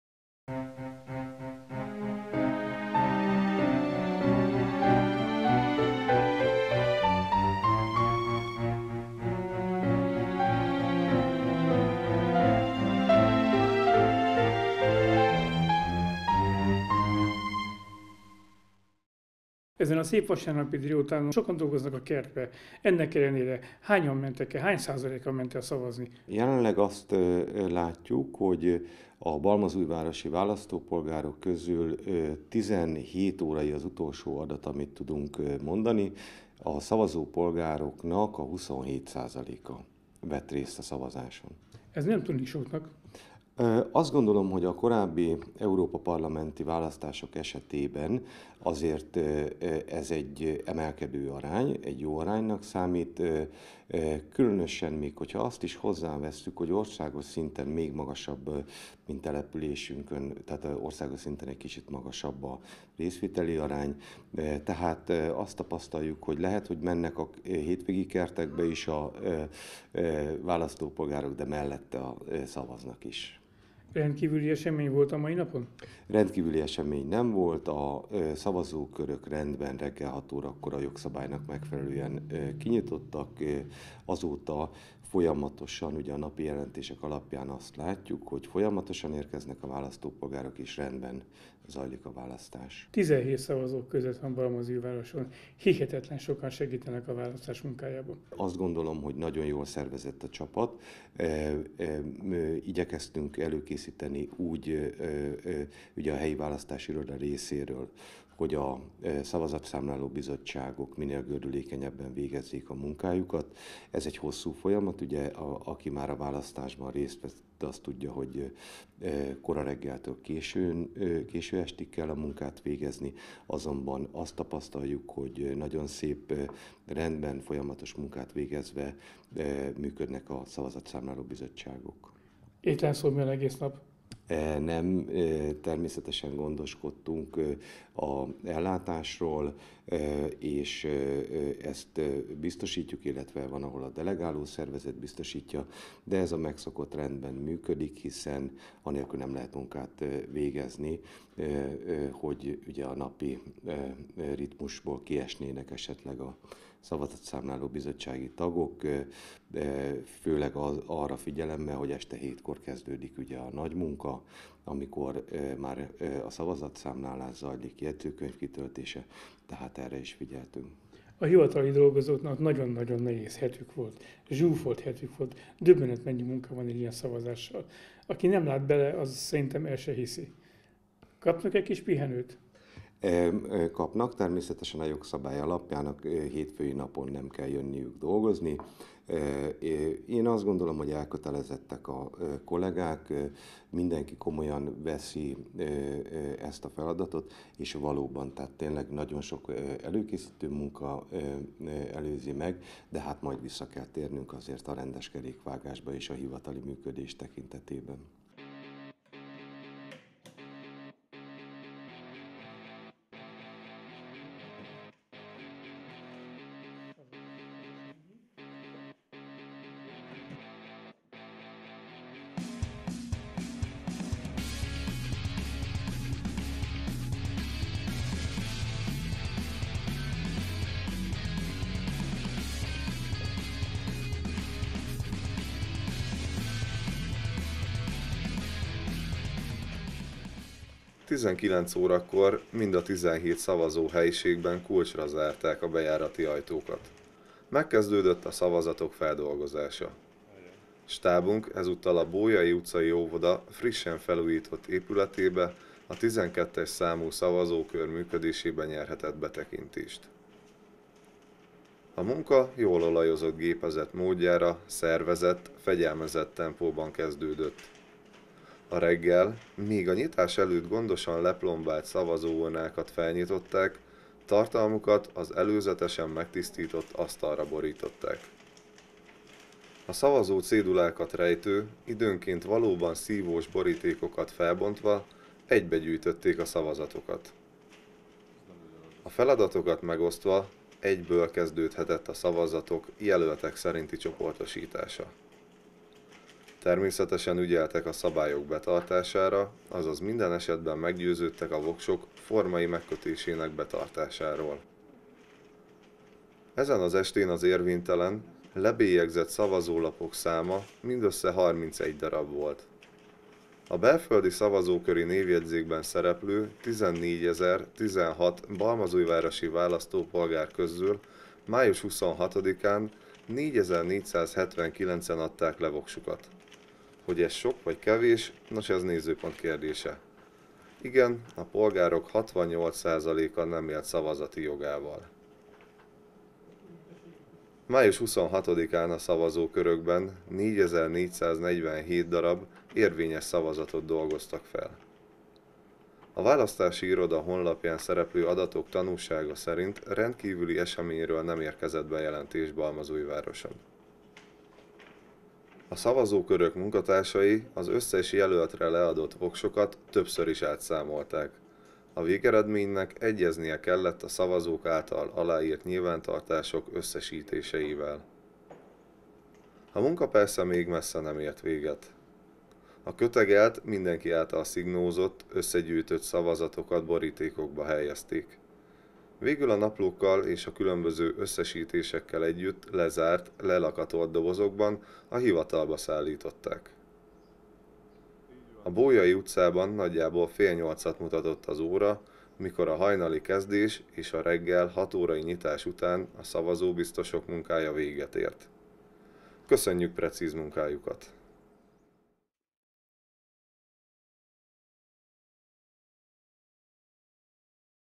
EP választás Balmazújvároson
Balmazújváros 17 szavazókörzetében a 2019-es EP választásokon május 26-án, a választópolgárok 31,96 százaléka adta le voksát.
A szavazati arányok nem sokban különböztek az országos trendektől.

A FIDESZ-KDNP      55.32%
DK                             13,33%
JOBBIK                    10,61%
MOMENTUM            6,45%
MSZP-PÁRBESZÉD  5,67%